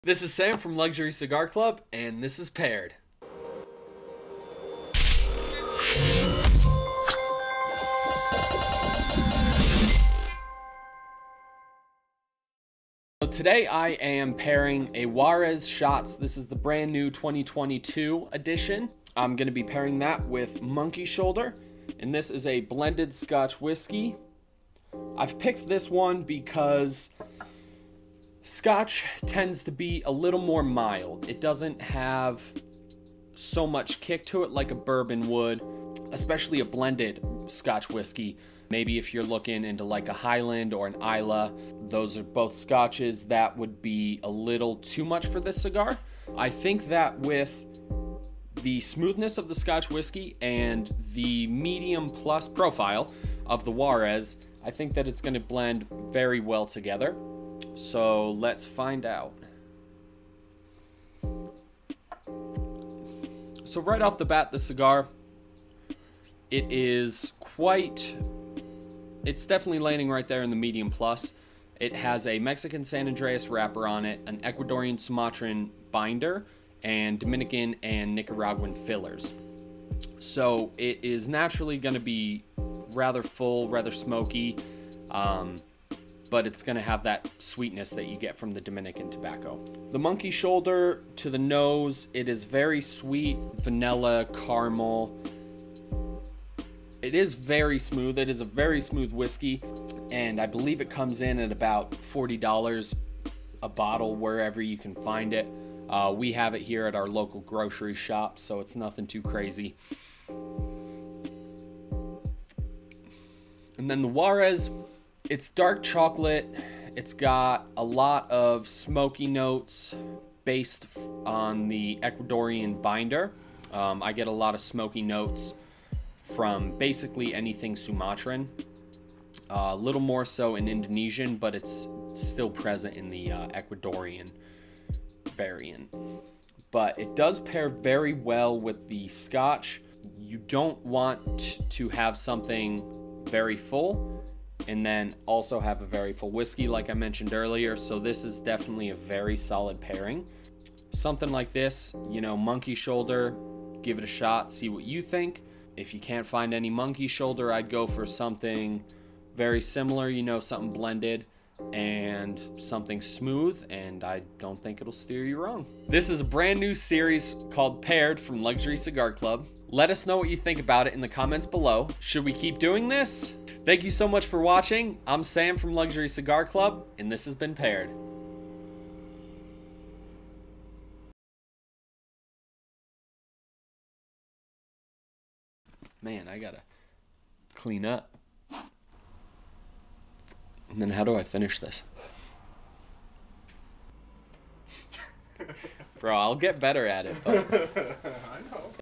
Paired Episode 1: Crowned Heads Juarez Shots 2022+ Monkey Shoulder
Paired Episode 1: Crowned Heads Juarez Shots 2022

Cigar - Crowned Heads Juarez Shots 2022

Rothschild 4”x50

Wrapper: Mexican San Andres

Binder: Ecuadorian Sumatra

Filler: Nicaraguan, Dominican

Retail: $5.95

Whiskey - Monkey Shoulder

Type: Blended Malt Scotch Whiskey

ABV: 43%

Retail: $31.99

Welcome to Paired, where we pick a cigar, and a spirit or beverage to enjoy together. This episode highlights two rather easy-to-find pairings. The brand new Crowned Heads Juarez Shots, and a blended scotch whiskey. Let us know what you think of this pairing, and if you have found any exceptional pairings yourself!